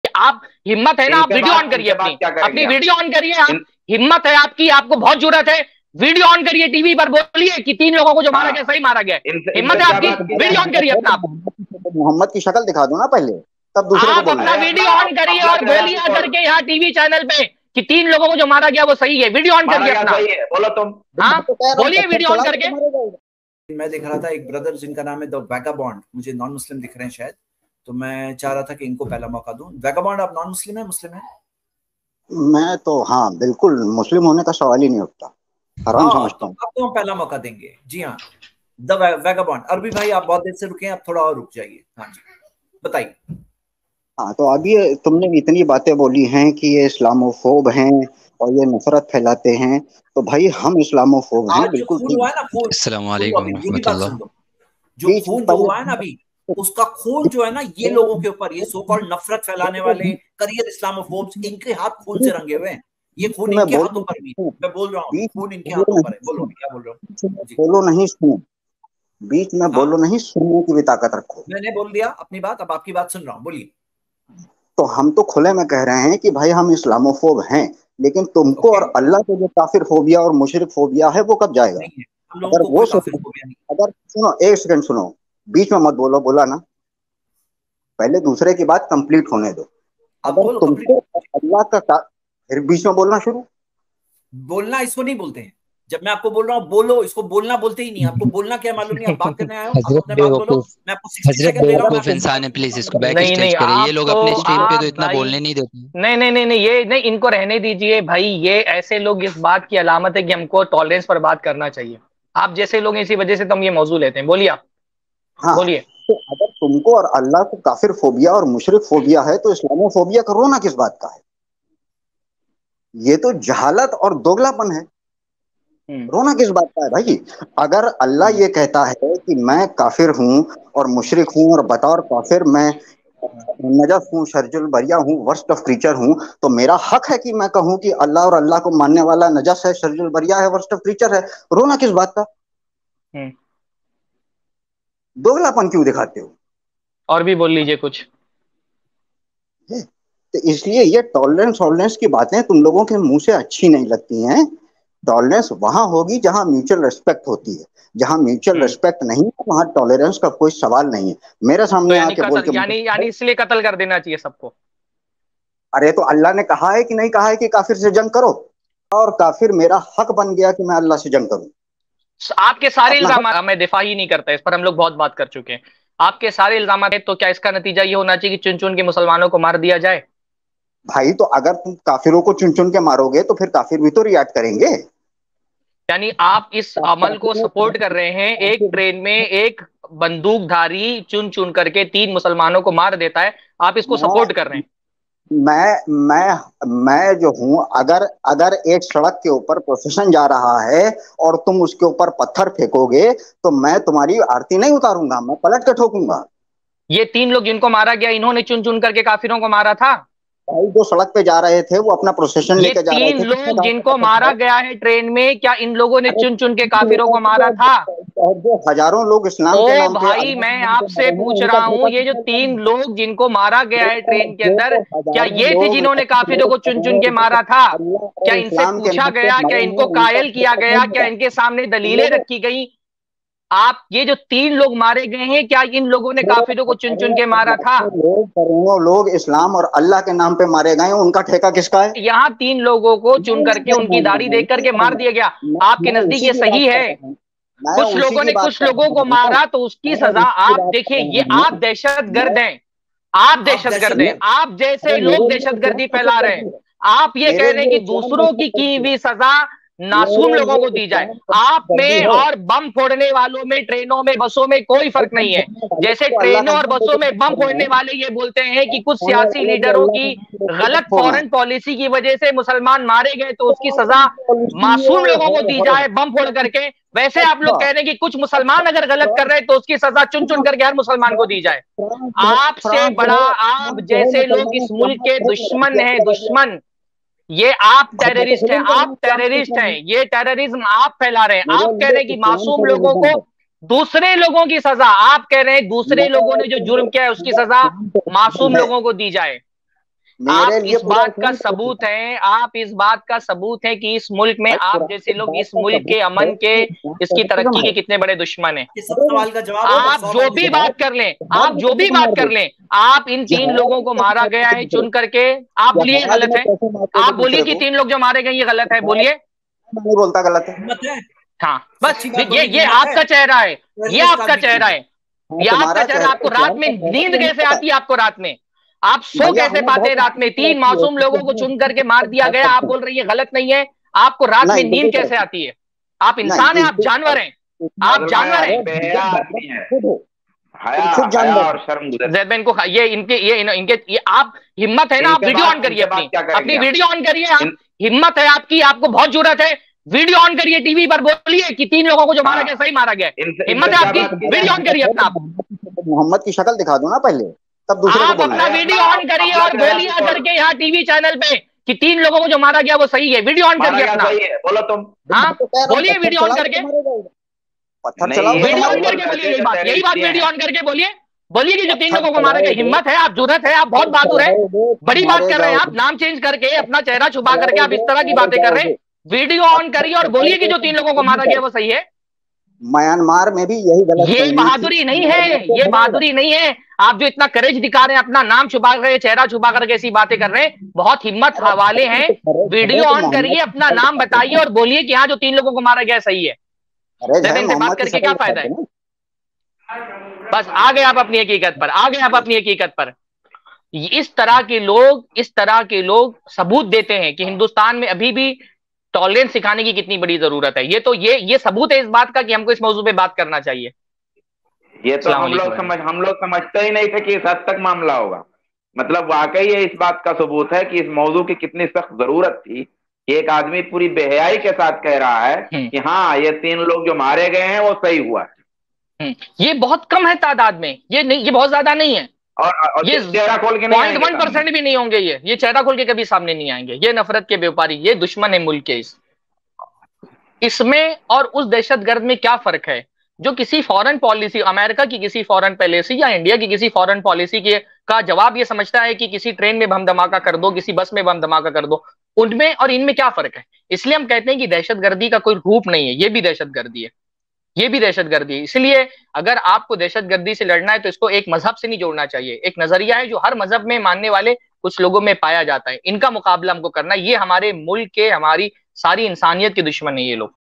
Train Mein 3 Logon Ki Hatya Ko Sahi Kehne Wala Shakhs Live Stream Mein by Sk Sarfaraz Faizi iPlu
Train Mein 3 Logon Ki Hatya Ko Sahi Kehne Wala Shakhs Live Stream Mein by Shaikh Sarfaraz Faizi iPlus TV